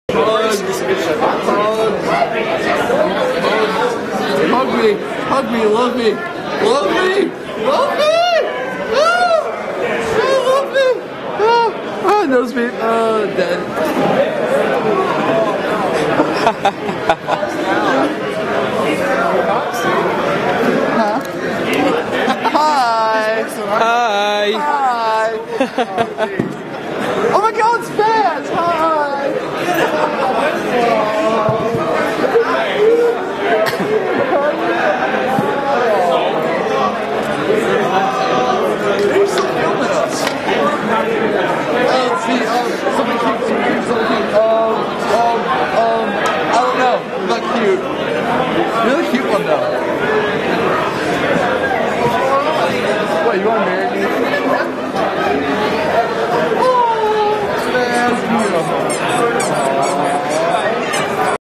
Midwest Furfest 2012 - Ace Fox Wants Hugs
Ace Fox wanting hugs in the lobby.